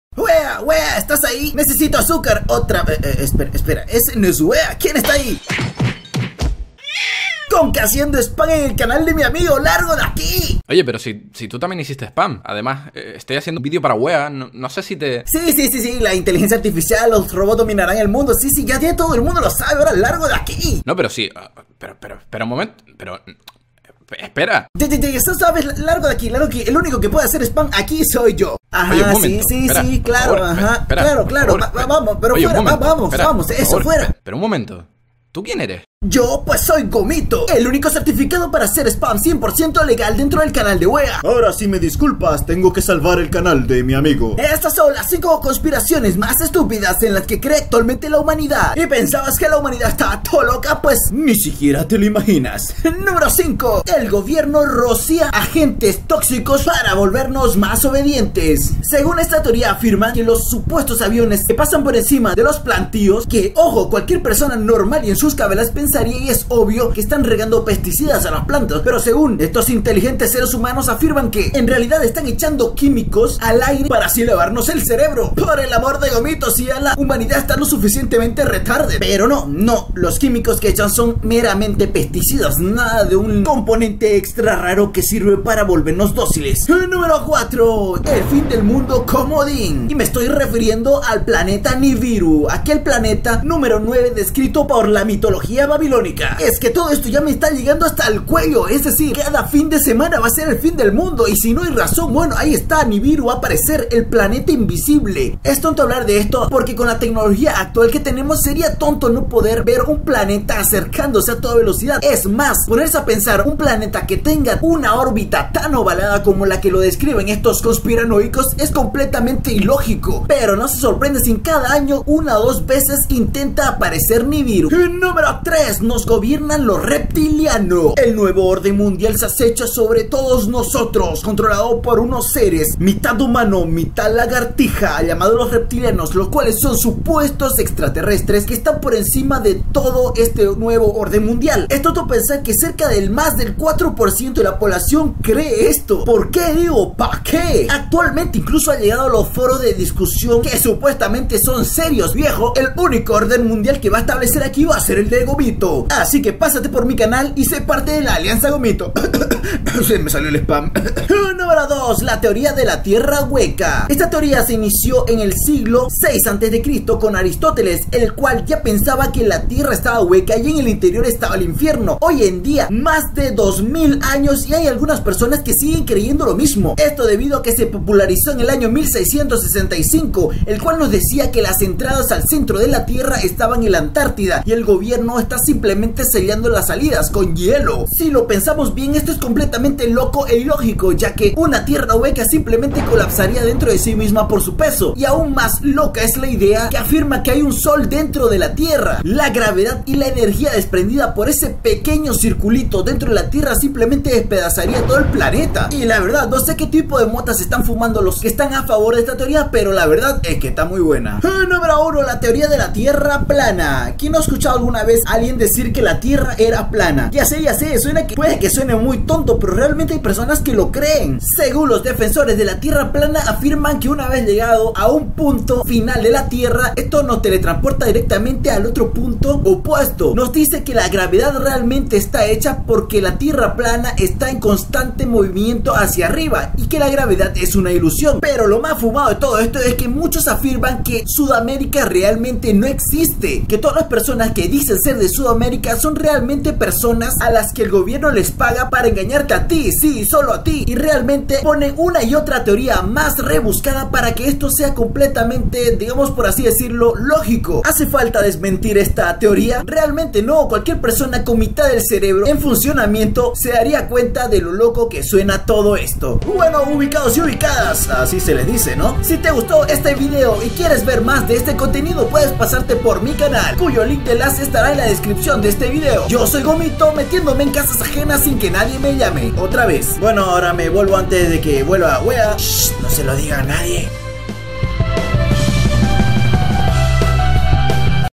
Las Conspiraciones Más Ridículas - Creidas HOY - Zona Gomito
Aunque la ciencia avanza, aún hay miles de personas que creen en conspiraciones.

Creditos por stock footage a CyberWebFX, Depock/MTSX , Free Stock Footage y Beachfront B-Roll